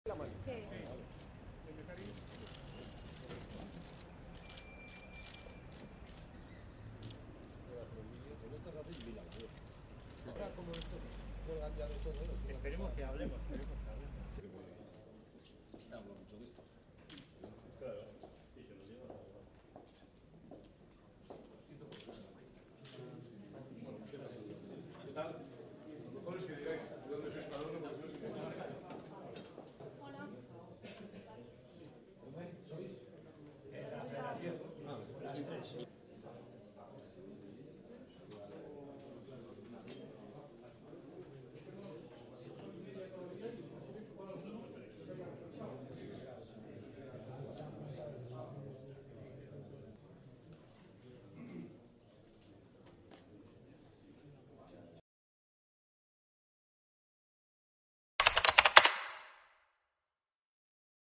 El Lehendakari se ha reunido con las asociaciones vecinales de Bizkaia
El Consorcio de Transportes está trabajando en una experiencia piloto con el billete único, ha explicado el Lehendakari

Con el fin de conocer de cerca sus problemas y preocupaciones, el Lehendakari ha mantenido un encuentro con 29 asociaciones vecinales de Bizkaia. Acompañado del Director de Participación Ciudadana, Txema Oleaga, el Lehendakari ha debatido sobre las inquietudes y propuestas que los ciudadanos querían transmitirle a través de sus representantes.